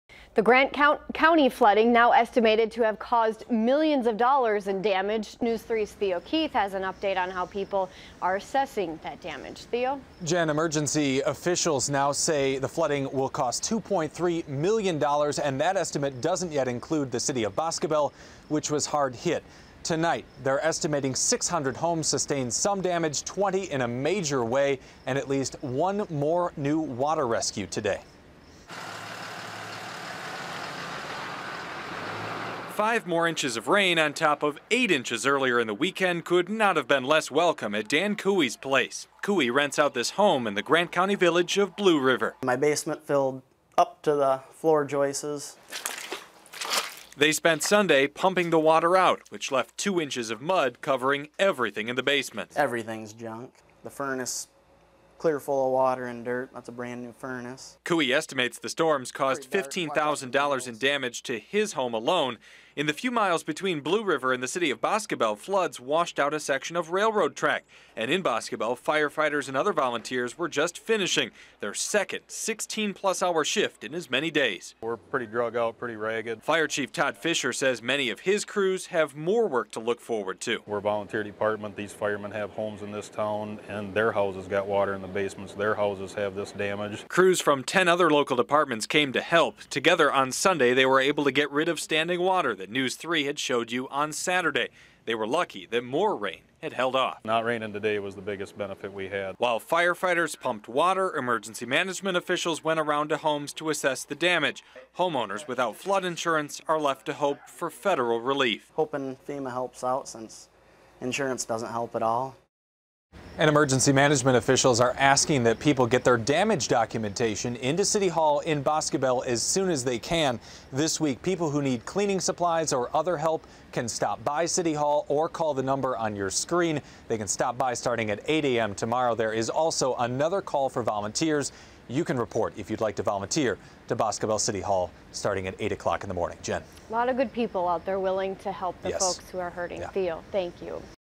Grant County flood repairs estimated to cost millions
Emergency officials estimate that repairs from the Grant County
flooding will cost $2.3 million. And that total doesn't yet include the
hard-hit city of Boscobel.